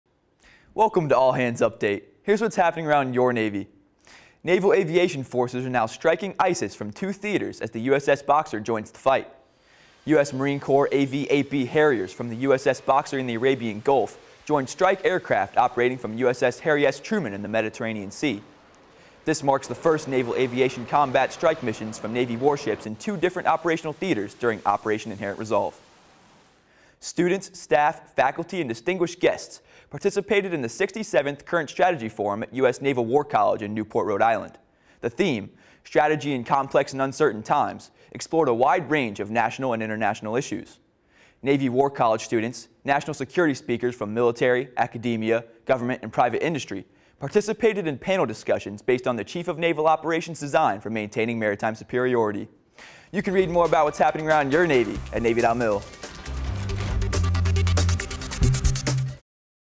Headlines for Friday, June 17, 2016 (HL-17)
USS Boxer joins fight against ISIS with USS Harry S. Truman. Students, staff, and distinguished guests attended the 67TH Current Strategy Forum at U.S. Naval War College.